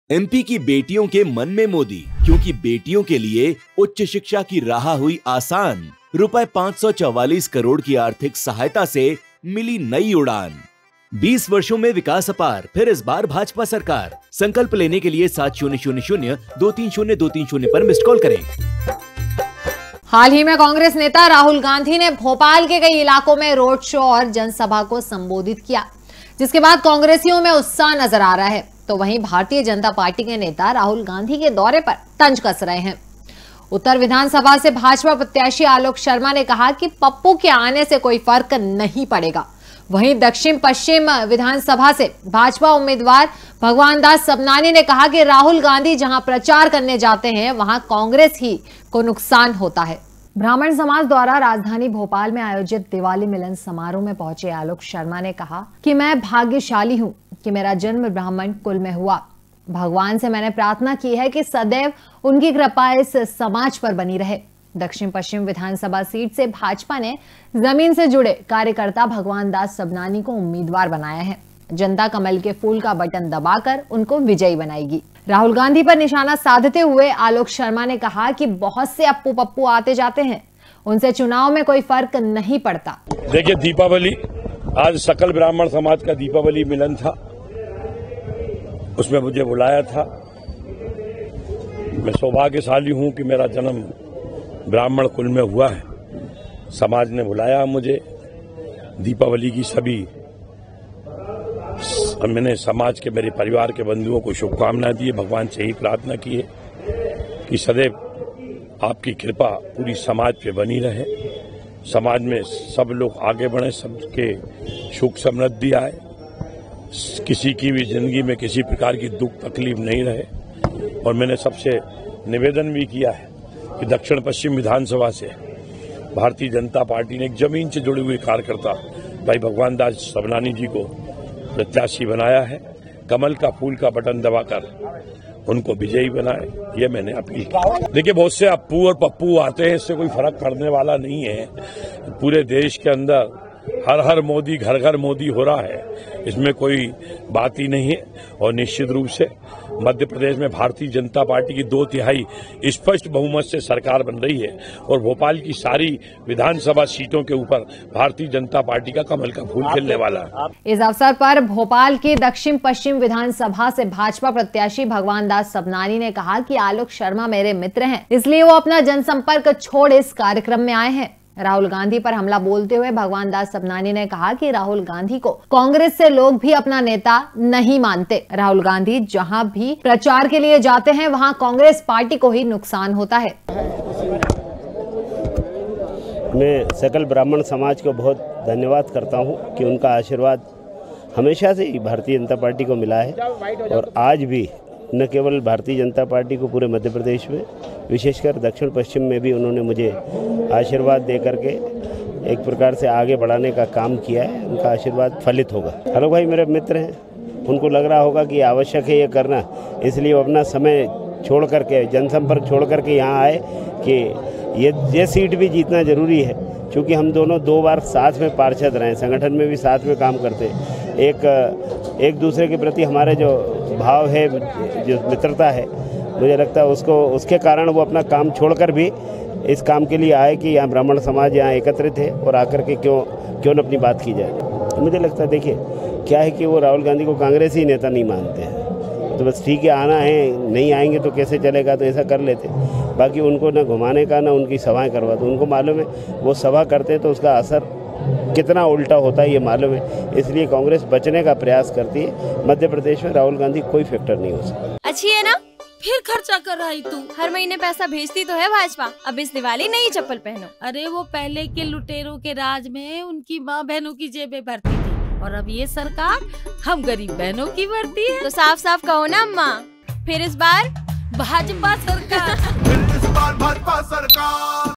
DAKHAL NEWS दिवाली मिलन समारोह में पहुंचे आलोक शर्मा
दिवाली मिलन समारोह में पहुंचे आलोक शर्मा
कांग्रेस नेता राहुल गाँधी पर कसा तंज